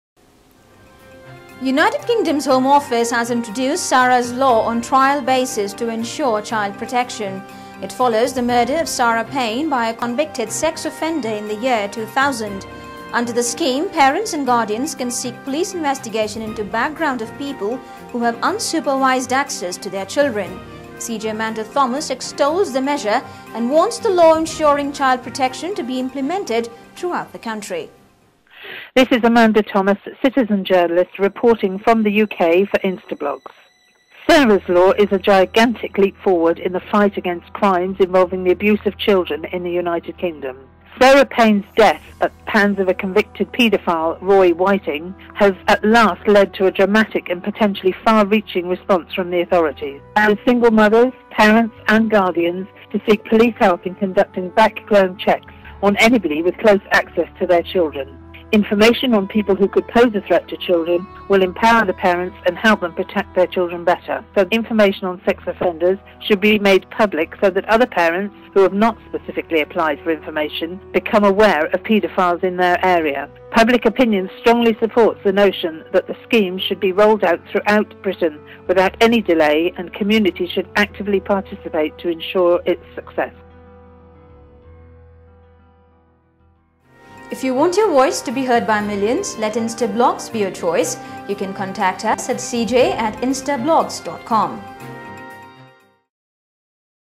Sarah Law
Britain introduces Sarah's Law to ensure child protection

Sarah's law is a gigantic leap in fighting crimes against children within United Kingdom. Finally Sarah Payne's death at the hands of a convicted pedophile Roy Whiting has led to dramatic response from public authorities. It allows single mothers, parents and guardians can seek police help in conducting background checks on somebody with close access to their children. Information on people who could pose a threat to the children's lives will empower the parents and help the latter protect their children better. However when compare to Megan's law in US the law falls short in one respect. It does not allow the information regarding convicted sex offender to be made public. This is unfair. The information on sex offenders should be made public so that other parents become vigilant towards freely roaming pedophiles that are a potential threat to their children. The scheme should be rolled out throughout Britain without any delay and communities should actively participate to ensure its success.